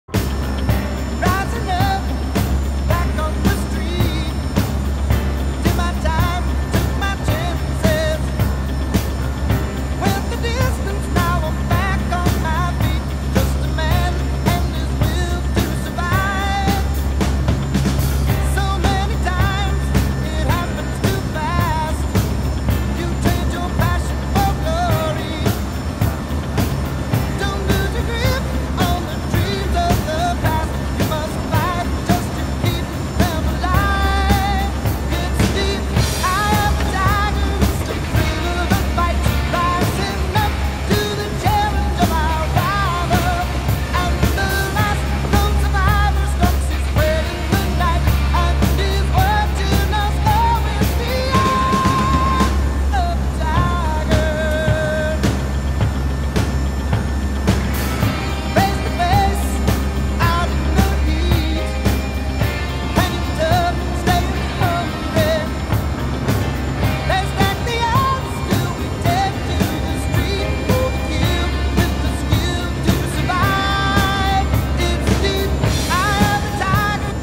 Inside Hurricane Matthew
A look inside Hurricane Matthew...and what are the pilots playing on board?